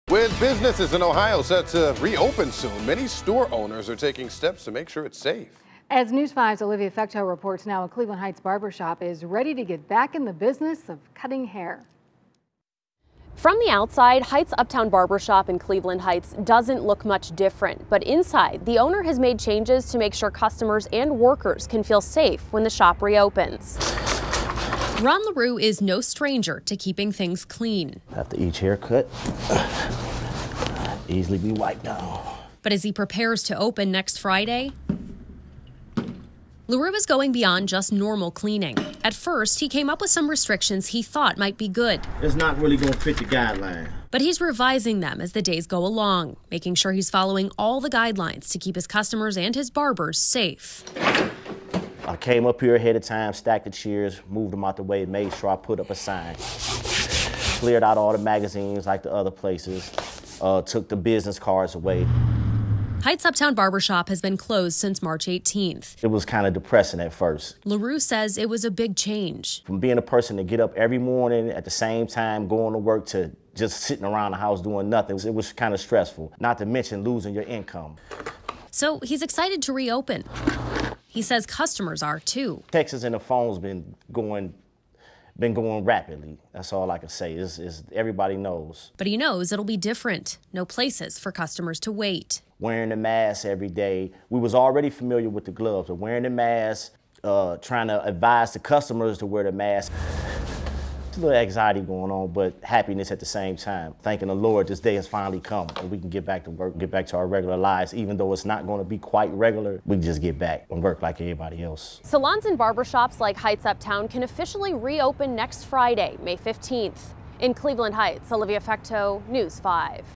Cleveland Heights barbershop gets ready to open; considering senior-only hours
With businesses in Ohio set to reopen soon, many store owners are taking steps to make sure it's safe for them to do so.